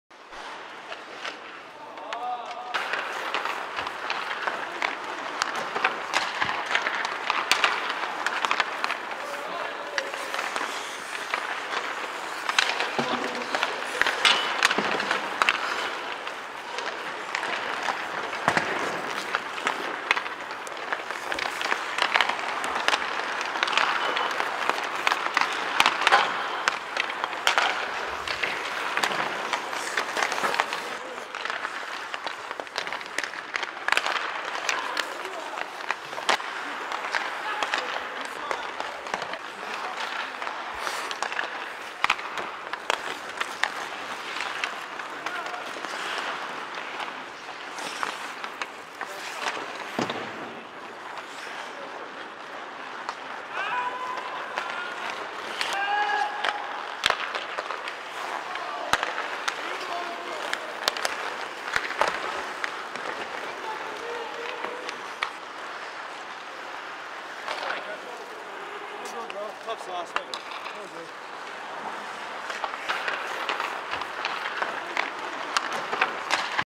Bruins morning skate in New Jersey 1/14
Some video from the Bruins morning skate in New Jersey at the Prudential Center ahead of opening night 1/14, courtesy of the Boston Bruins.